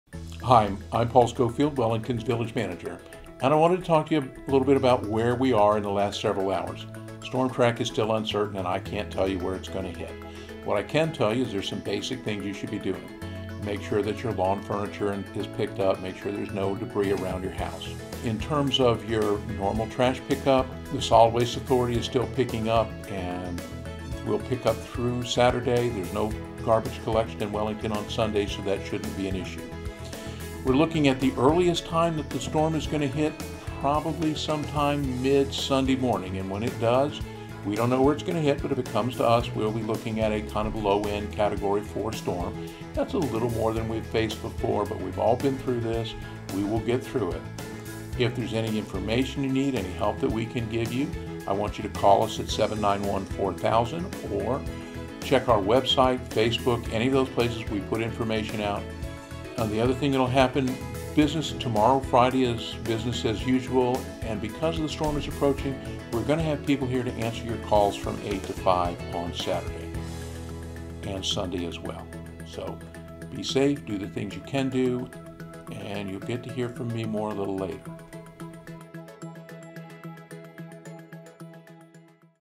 Hurricane Dorian Wellington Update 2 Village Manager Paul Schofield
Get the latest update from Wellington's Village Manager on Hurricane Dorian.